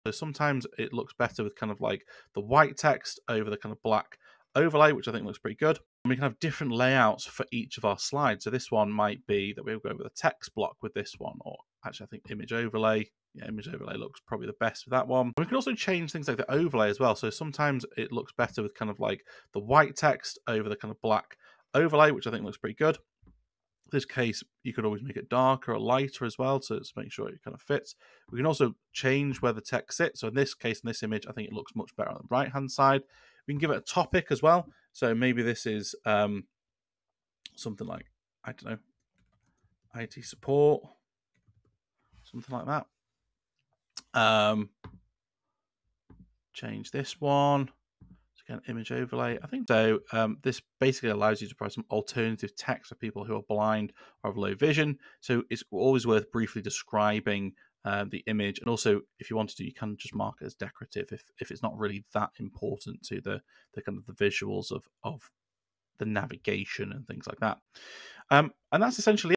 SharePoint Image Overlay Tips: Make Your Content Pop!
Discover how to master image overlays, layouts, and accessibility options in SharePoint! In this tutorial, we will guide you through adjusting image overlays, changing layouts, and optimizing for accessibility to enhance your SharePoint pages. Learn tips for republishing and ensuring your content shines!